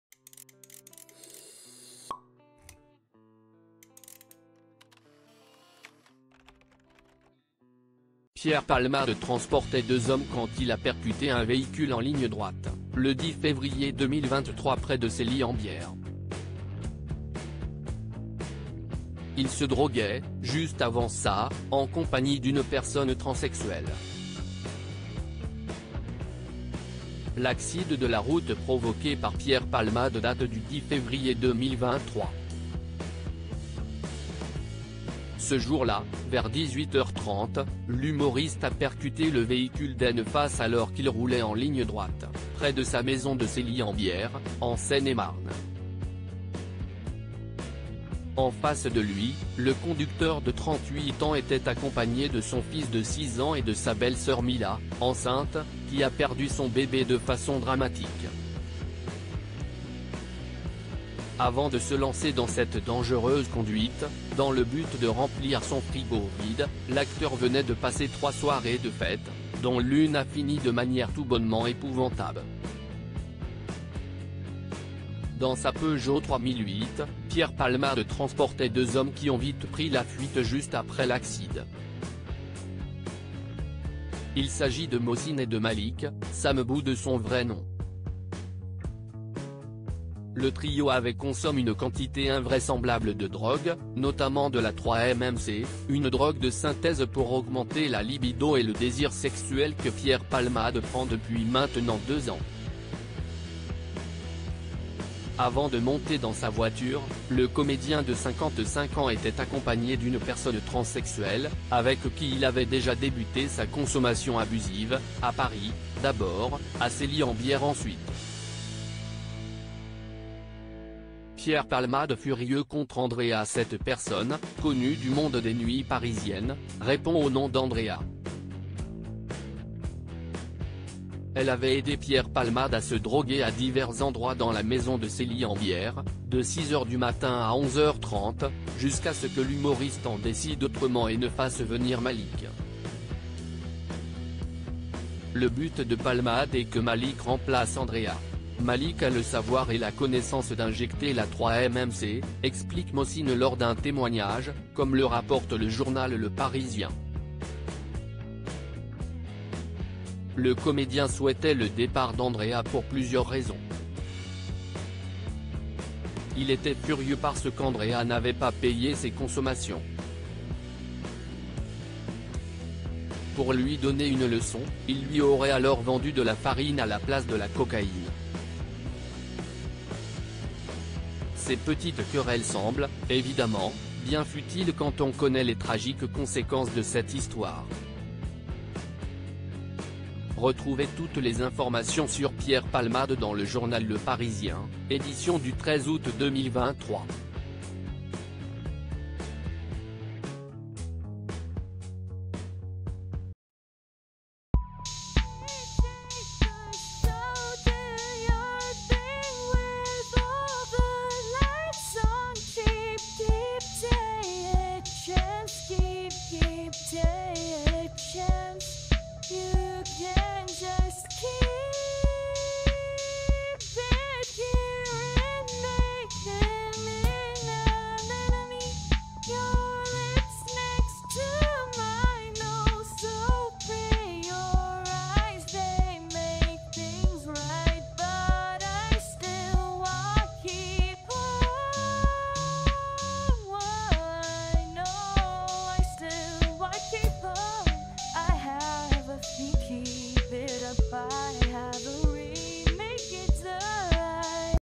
Accident de Pierre Palmade L'acteur a tenté de punir une personne transexuelle avant le carambol
Accident de Pierre Palmade : L'acteur a tenté de punir une "personne transexuelle" avant le carambolage